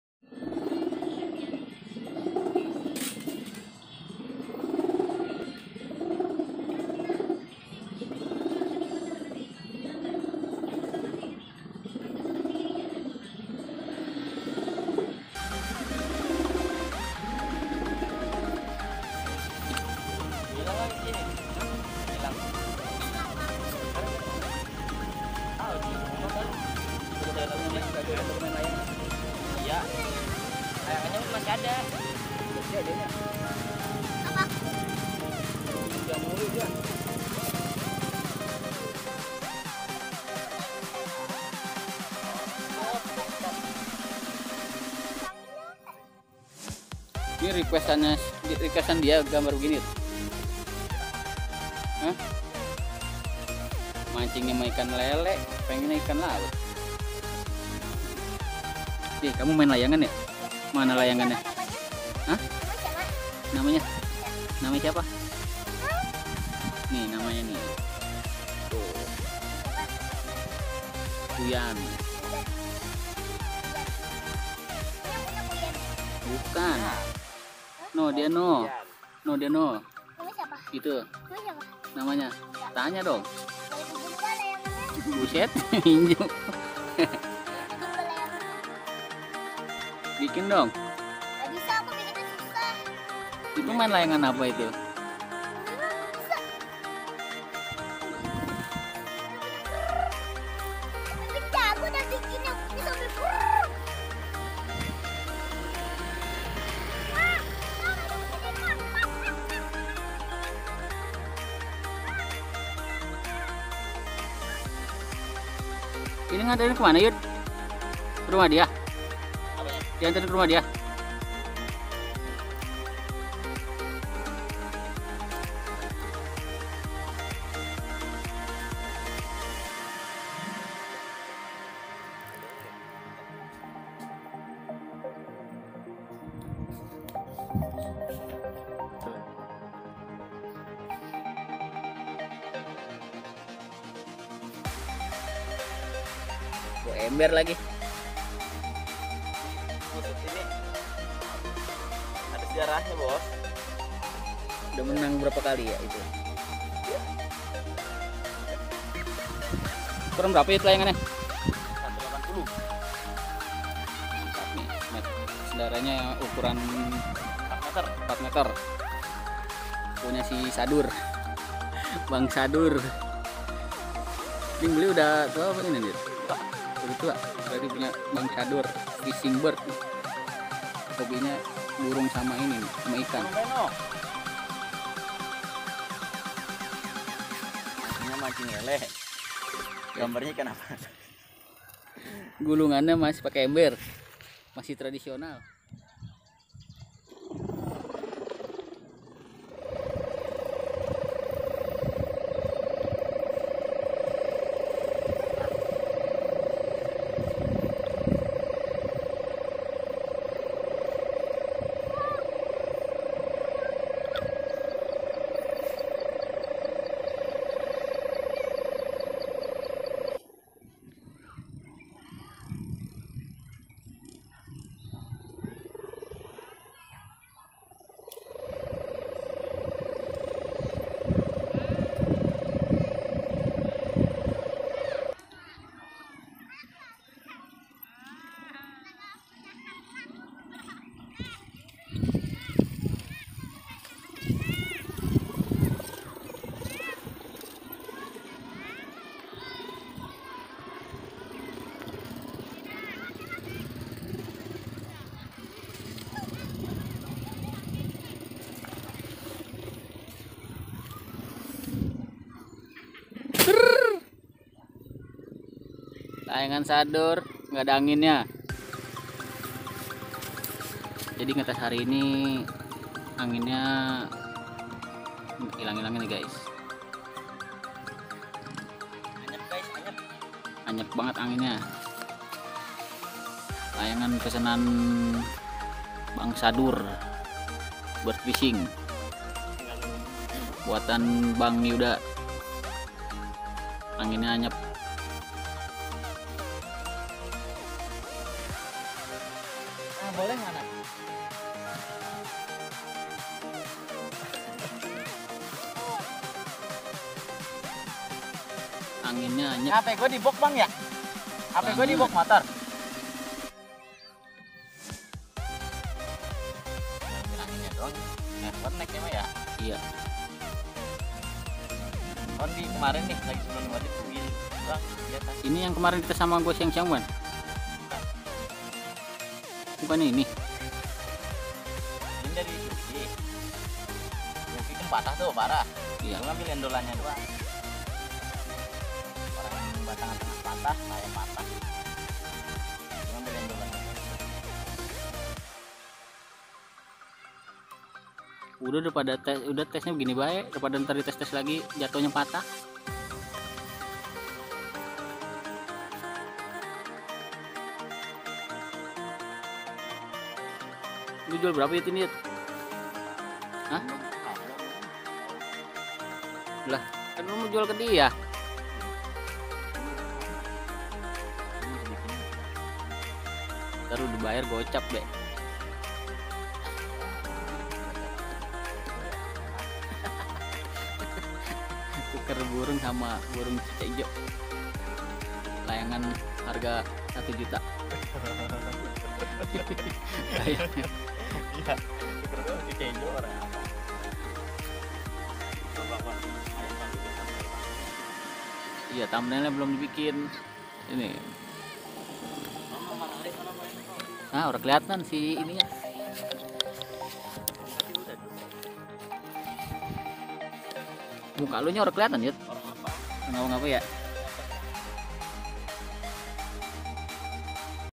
TES GAPANGAN 1.8M SENDAREN 4M ‼️BANG YUDA TANGSEL
Tes Perdana Gapangan Pegon Di Lapangan Andalass Tangsel

Biar Gak Ketinggalan Video Serunya
Dan Banyak Lagi Video Berikutnya
Terima Kasih.

FOLOW INSTAGRAM
tangsel_kite